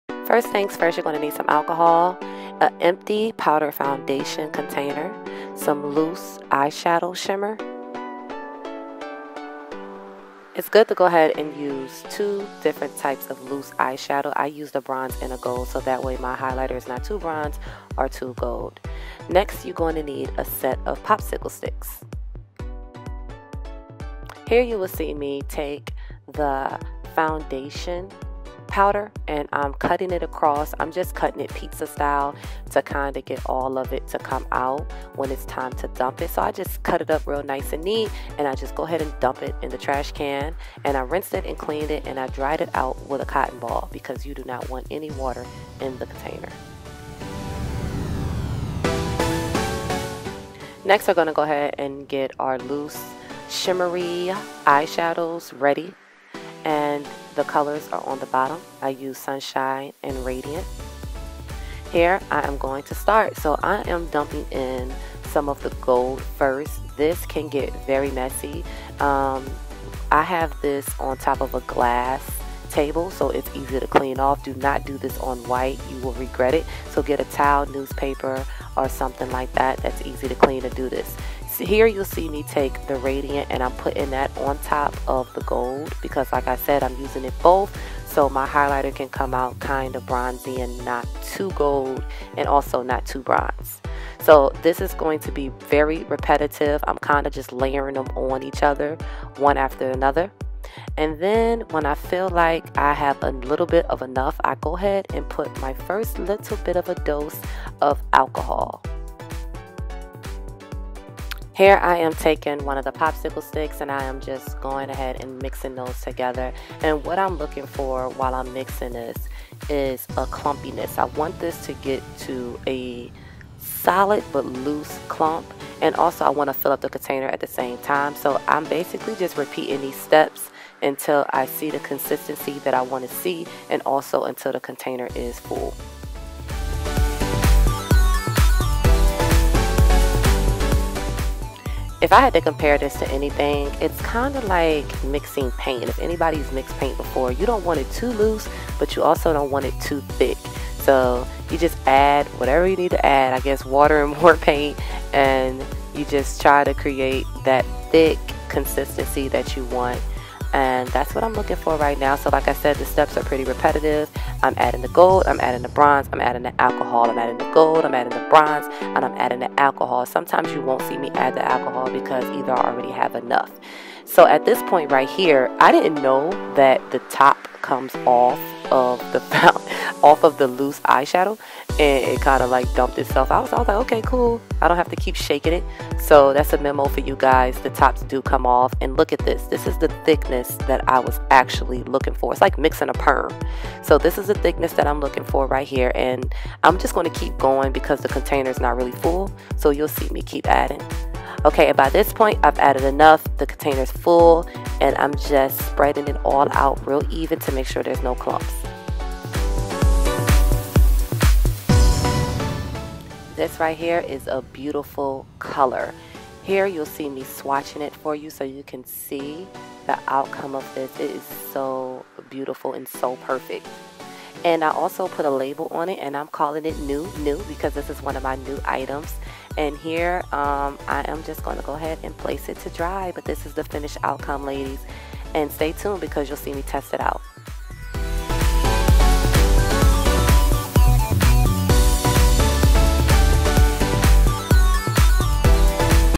DIY CREATE A HIGHLIGHTER UNDER $10
DIY TIME! CREATE YOUR VERY OWN HIGHLIGHTER NO MORE $60 HIGHLIGHTERS ! MAKE YOUR OWN UNDER $10 !

❤️Spam This Video With "Likes"

📝PRODUCTS:

ALCOHOL
L.A COLOR SHIMMER EYESHADOW
POPSCILE STICKSPOWDER FOUNDATION CONTAINER
POPSICLE

🗣SHOUT OUTS!

SnapChat👻